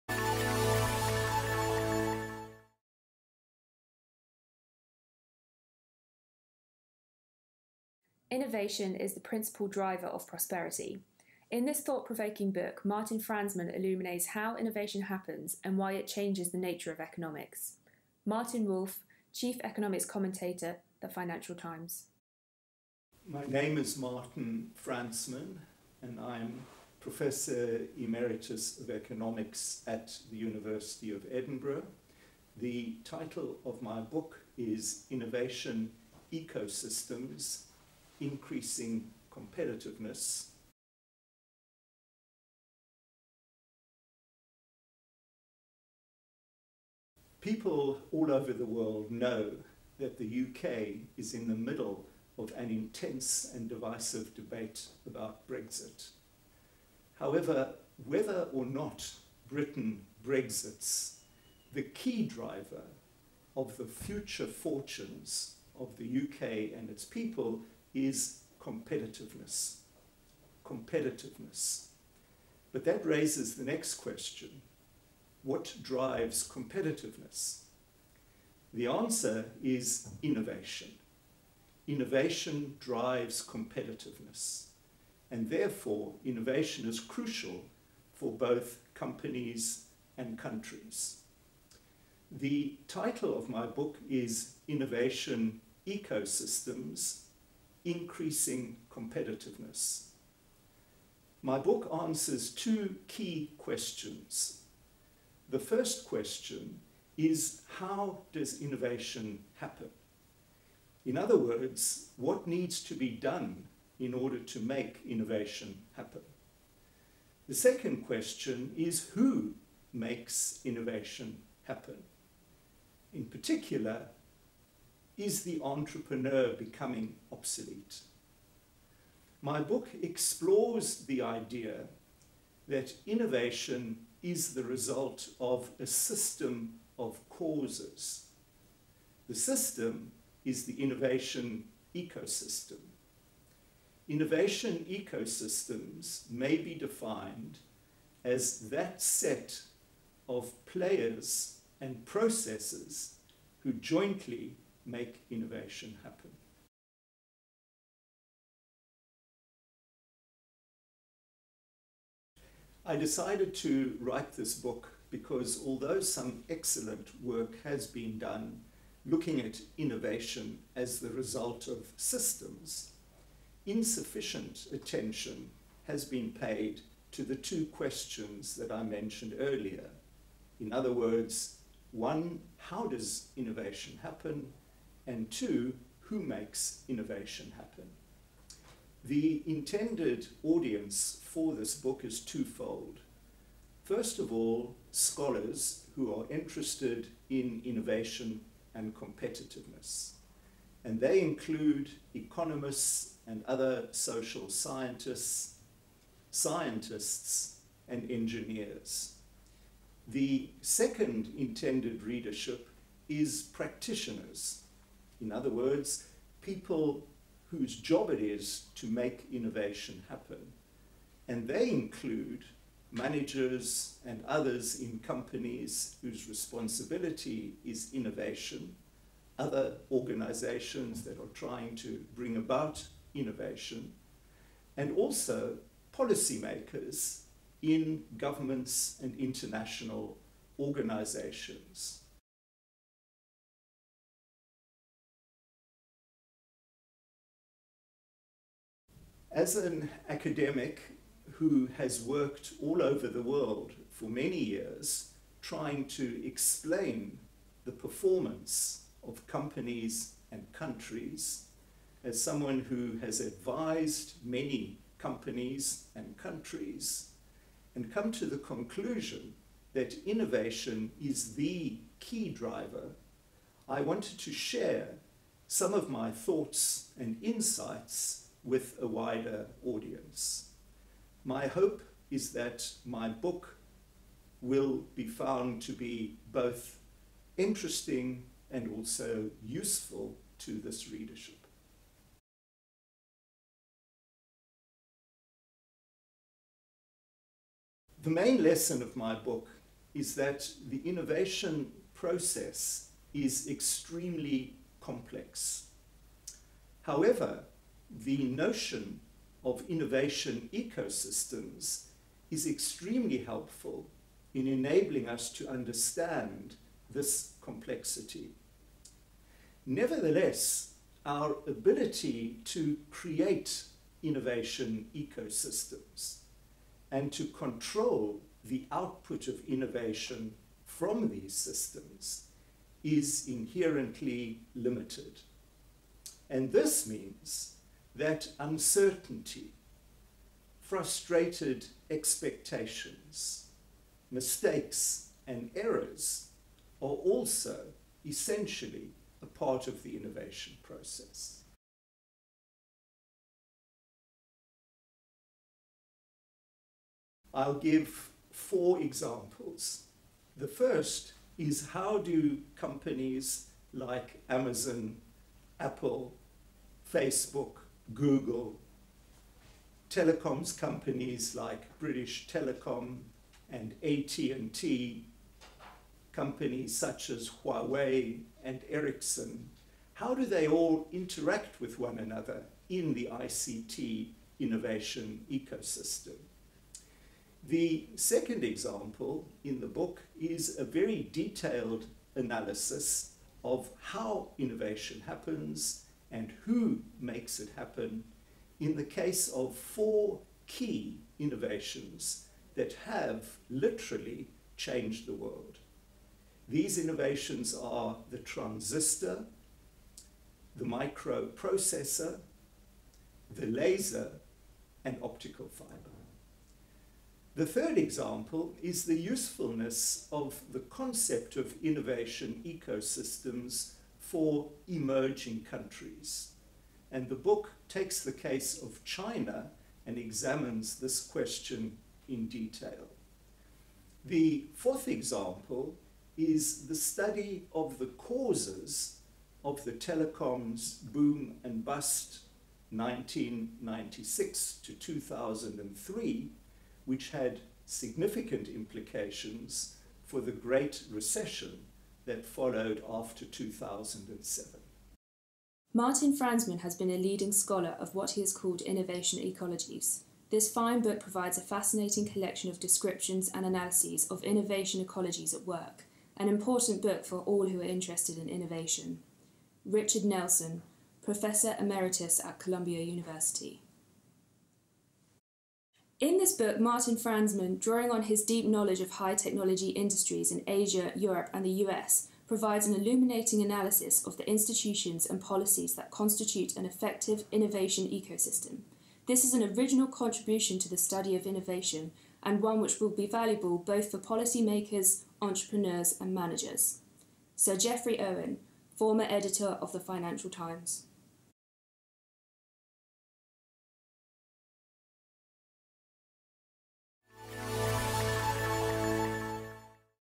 Interview with Martin Fransman, author of Innovation Ecosystems
Martin Fransman presents a new approach to understanding how innovation happens, who makes it happen, and the helps and hindrances. Looking at innovation in real-time under uncertainty, he develops the idea of an 'innovation ecosystem', i.e. a system of interrelated players and processes that jointly make innovation happen. Examples include: how companies like Amazon, Google, Facebook, Apple, AT&T, and Huawei interact in the ICT Ecosystem; four innovations that changed the world - the transistor, microprocessor, optical fibre, and the laser; the causes of the telecoms boom and bust of the early 1990s that influenced the Great Recession from 2007; and the usefulness of the idea of innovation ecosystems for Chinese policy makers. By delving into the complex determinants of innovation this book provides a deeper, more rigorous understanding of how it happens. It will appeal to economists, social scientists, business people, policy makers, and anyone interested in innovation and entrepreneurship.